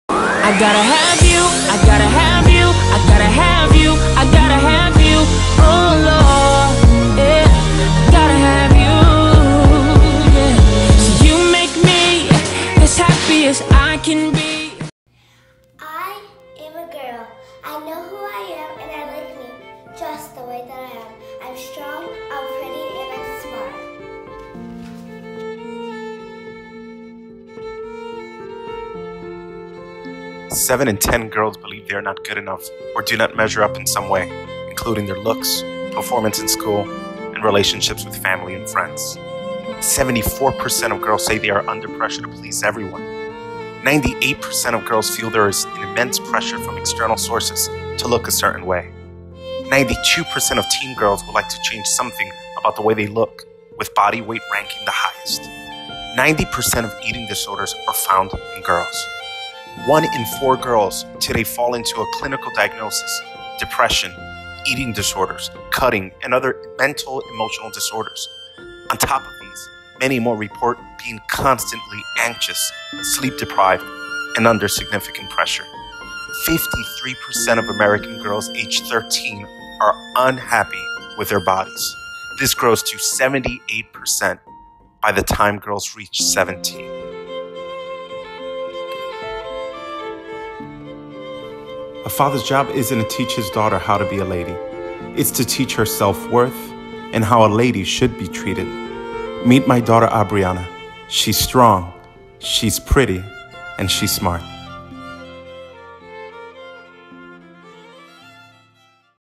I'm A Girl And I Like Me Just The Way I Am
Our daughters face so much pressure.  That's why it's important for father's to recognize that a father's job isn't to teach his daughter how to be a lady.  It's to teach his daughter self-worth, and how a lady should be treated.  Meet Abriana.  She's strong, pretty and smart!